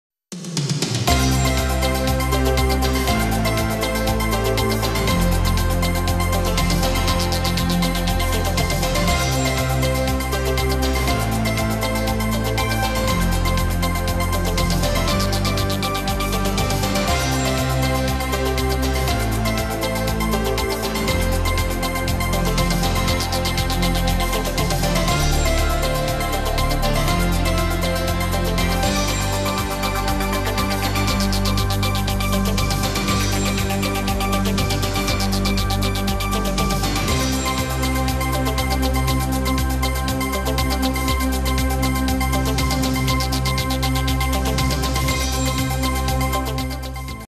【2014.05.16】國際股市 -udn tv
提供歐美股市2014/05/15收盤指數，以及亞洲各股市2014/05/16盤中指數(更新時間如影片所示，非收盤時指數)。完整內容 , FB , Twitter , G+